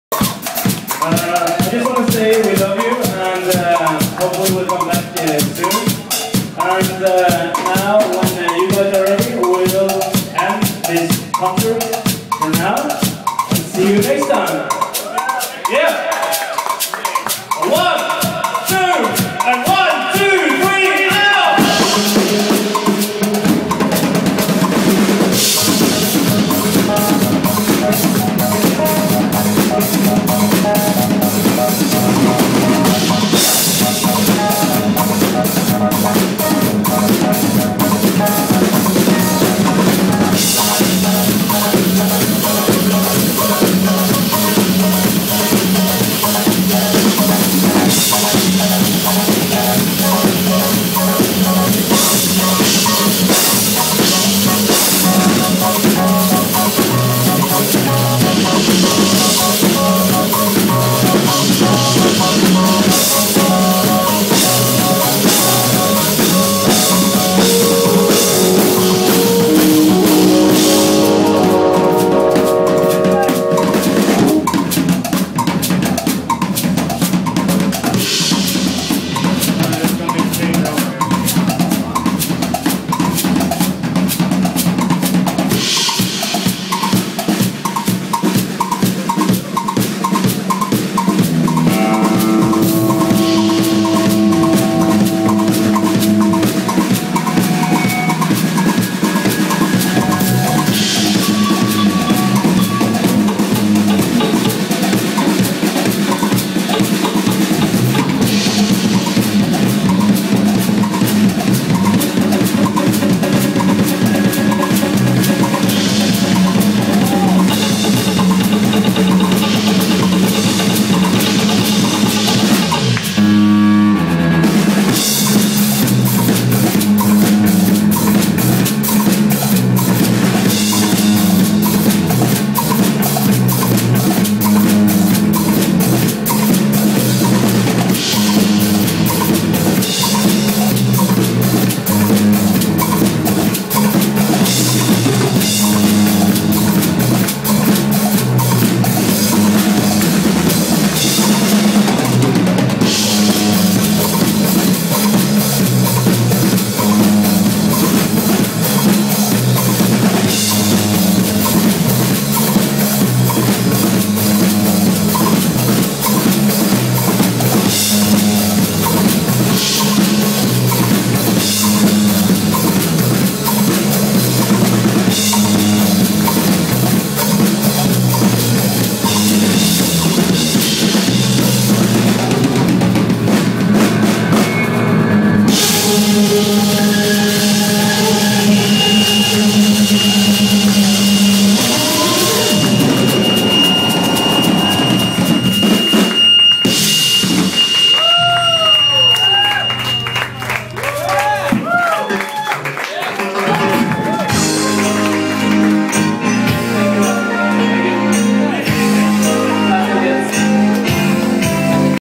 Casiokids, Set Closer, Live @ Kung Fu Necktie Philadelphia 101711
Casiokids close out their set at KFN by all switching instruments and jamming out.